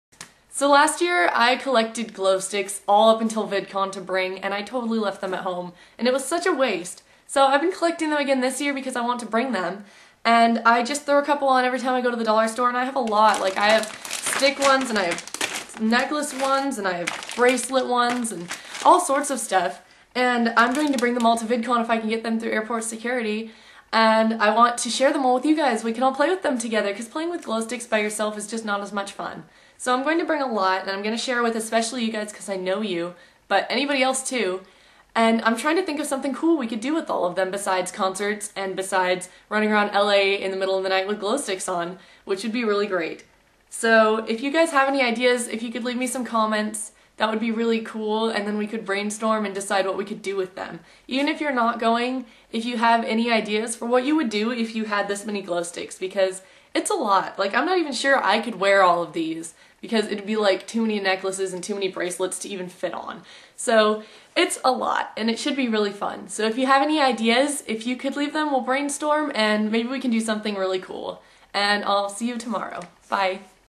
Vlog to Vidcon 14: Glowsticks!
We're going to pretend I didn't just miss three days of vlogging. Moving on...

I'm bringing all these glowsticks, so we need to find something awesome to do with them! Otherwise I'll feel awkward trolololol.